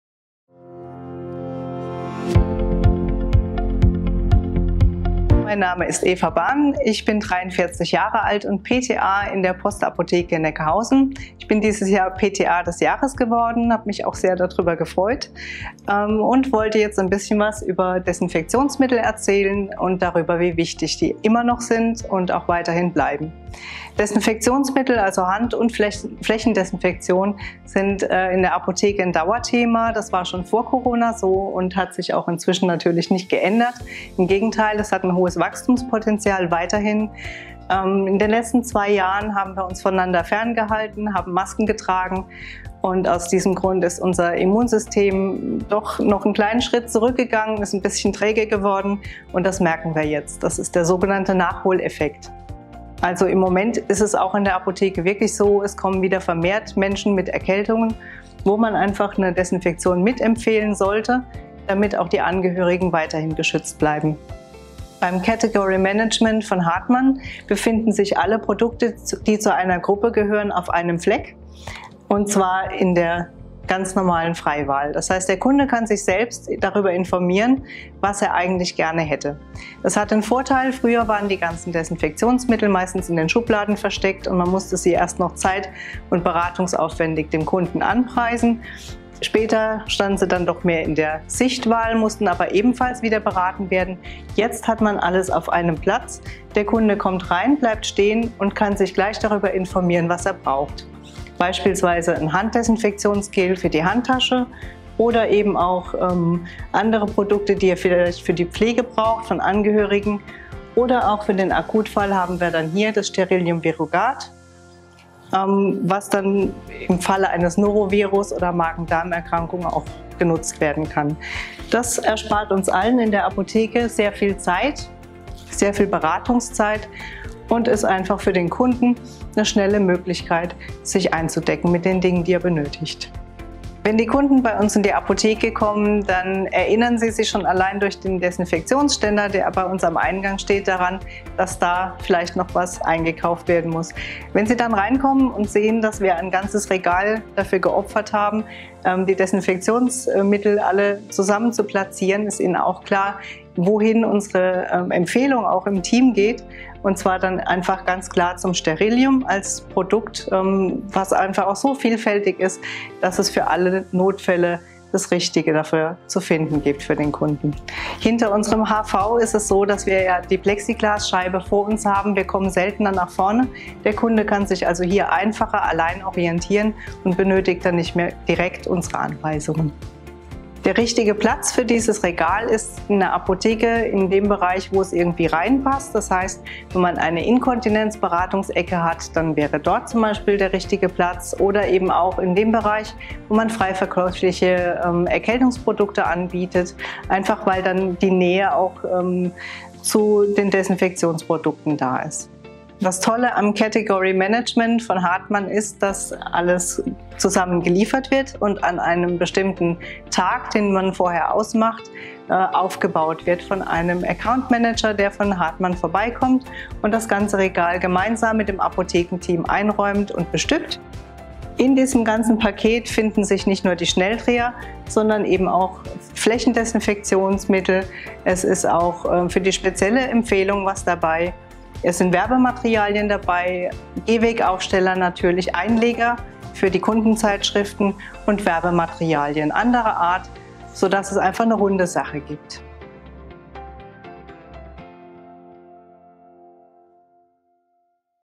Sterillium®: Category Management in der Apotheke - PTA des Jahres 2021 Eva Bahn stellt Vorteile vor
Frau Eva Bahn, Pharmazeutisch-technische Assistentin (PTA) des Jahres 2021, erklärt im Video die Vorteile des Category Managements in der Apotheke für Sterillium®.

Category oder auf Deutsch Warengruppenmanagement in der Apotheke ist die sinnvolle Gruppierung und Präsentation von Waren, um Kundinnen und Kunden eine bessere Orientierung zu geben. Durch den Fokus auf Produkte einer bestimmten Kategorie finden Kundinnen und Kunden auf Anhieb das, was sie brauchen. Das steigert die Kundenzufriedenheit und den Abverkauf zugleich.